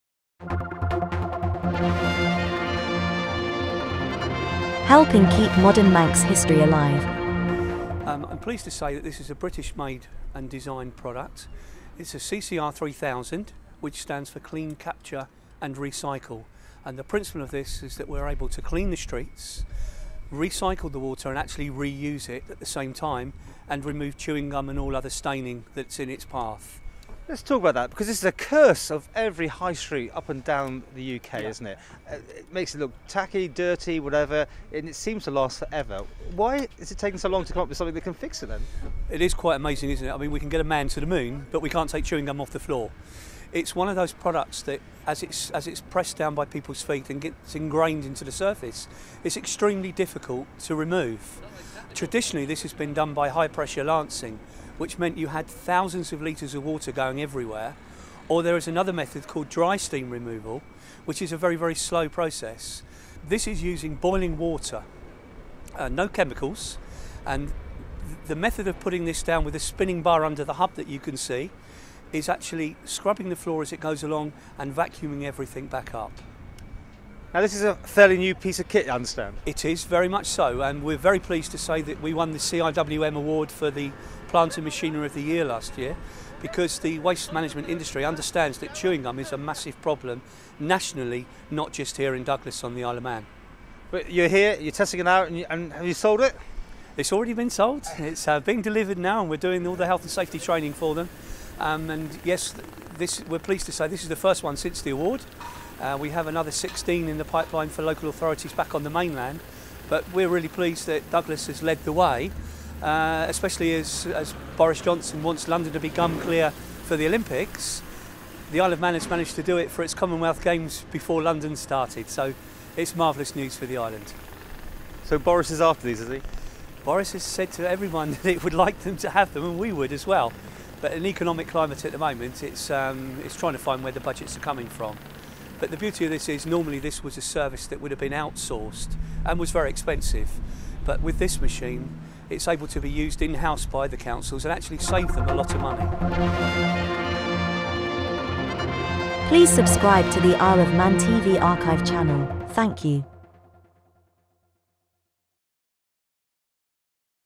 Chewing gum to be blasted off Douglas streets: 18.2.2011
Douglas council invested in a new machine which claimed to remove chewing gum from pavements in an energy efficient and environmentally friendly way using high pressure, high temperature, water on pavements to remove chewing gum. Gum Clear managing director Martin Whitbread explained more to Paul Moulton in February 2011.